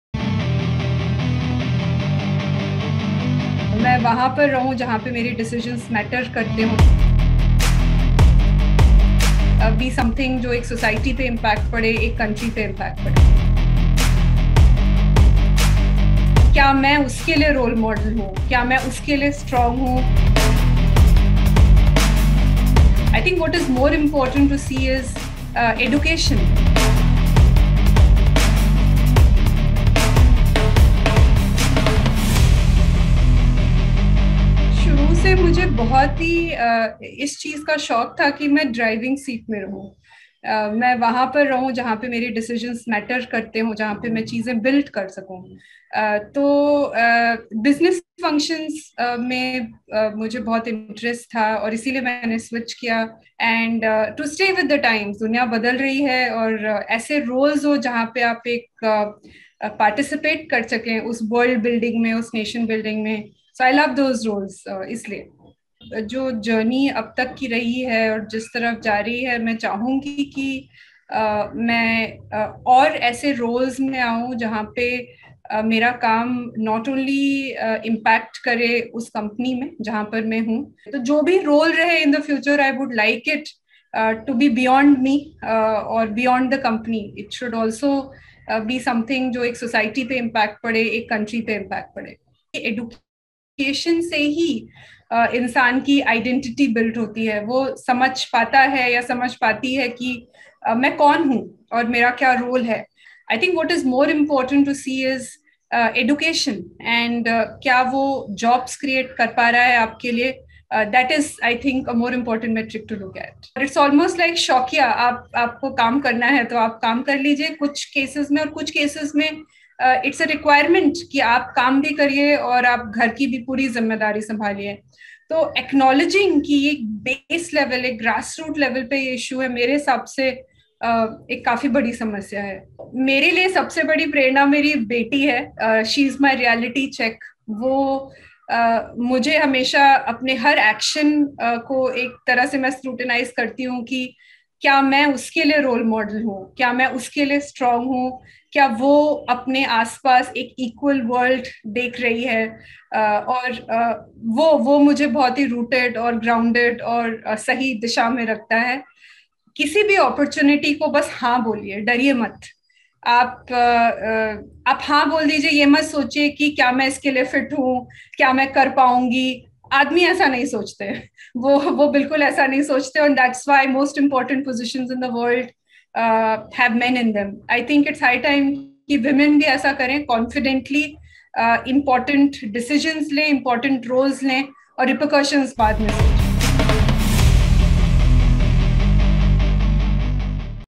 NB Learning Vibes: Learn From The Expert: Pali Tripathi
NBTheLearningVibes

किसी भी अपॉर्चुनिटी को हां बोलिए, डरिए मत। इंपॉर्टेंट डिसीजन लें, इंपॉर्टेंट रोल लें। नतीजों के बारे में बाद में सोचिए। - पाली त्रिपाठी

आप भी इन एक्सपर्ट्स की तरह अपनी जिंदगी को नई राह दे सकते हैं । तो जरूर देखें The Learning Vibes और नवभारत के साथ आगे बढ़ें।